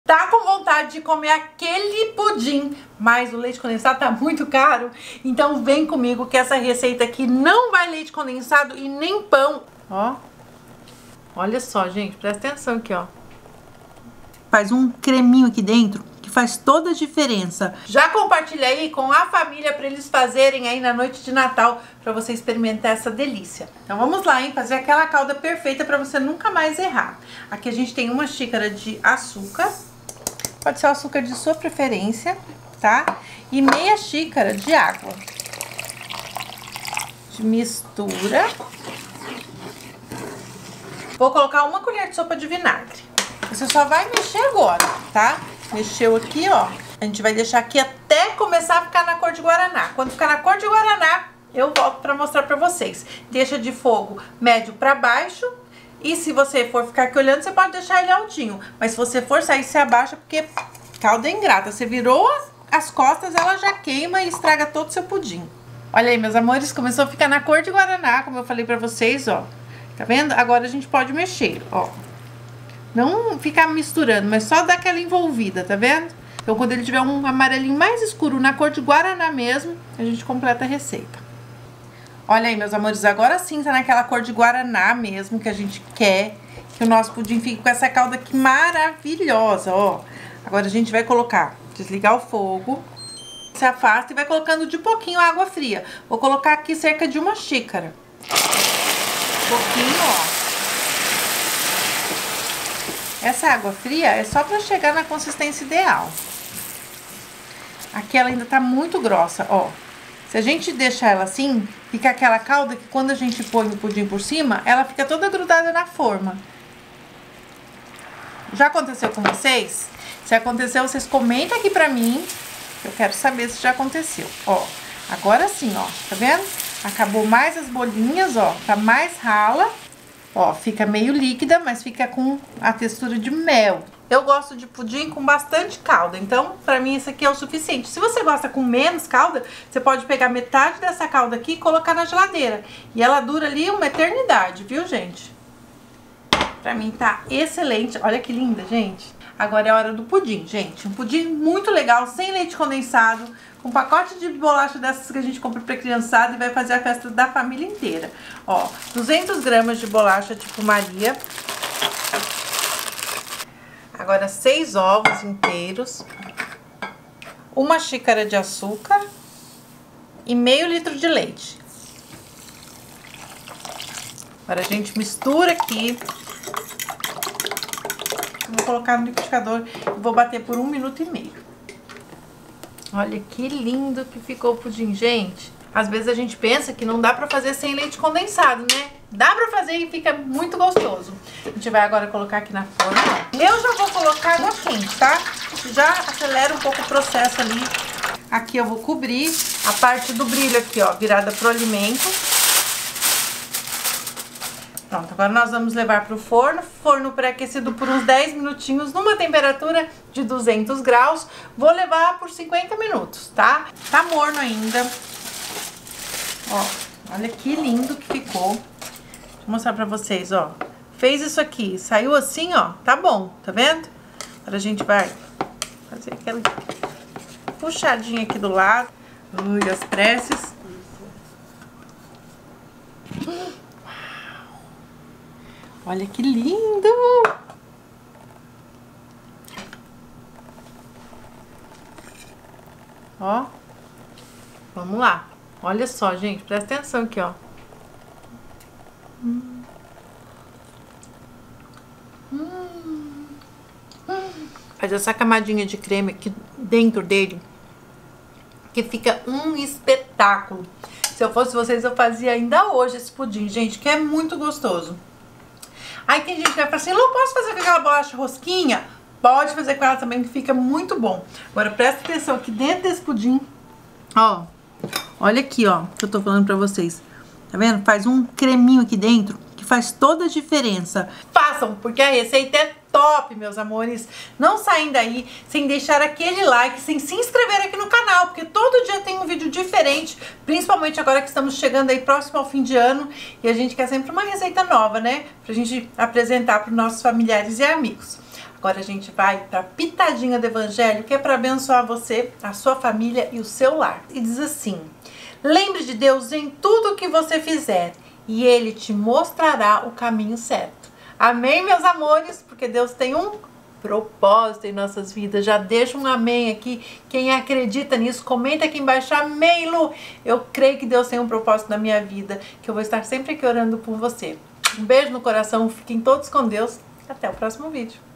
PUDIM SEM PÃO E SEM LEITE CONDENSADO - Luciana Mirihad
PUDIM COM UM TOQUE ESPECIAL PARA ECONOMIZAR NO LEITE CONDENSADO

Calda:
1 xícara (chá) de açúcar
Meia xícara (chá) de água
1 colher (sopa) de vinagre

Pudim:
1 xícara (chá) de água fria
200g de bolacha maria
6 ovos inteiros
1 xícara (chá) de açúcar
Meio litro de leite integral